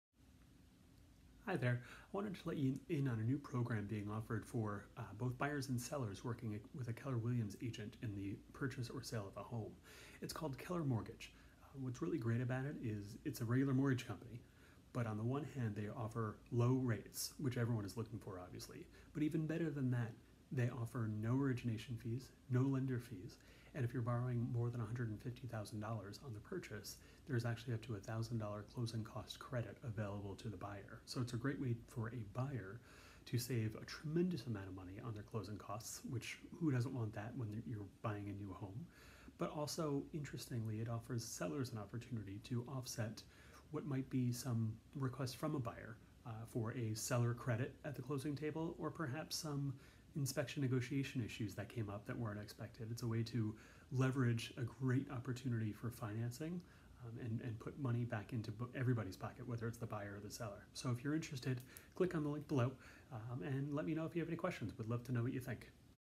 Keller Mortgage
Keller Mortgage offers home buyers and sellers the opportunity to save thousands, through a financing program available exclusively to people working with a Keller Williams agent.  Low rates, no lender or origination fees, and a $1,000 credit at closing means more money in your pocket.  It's a win-win!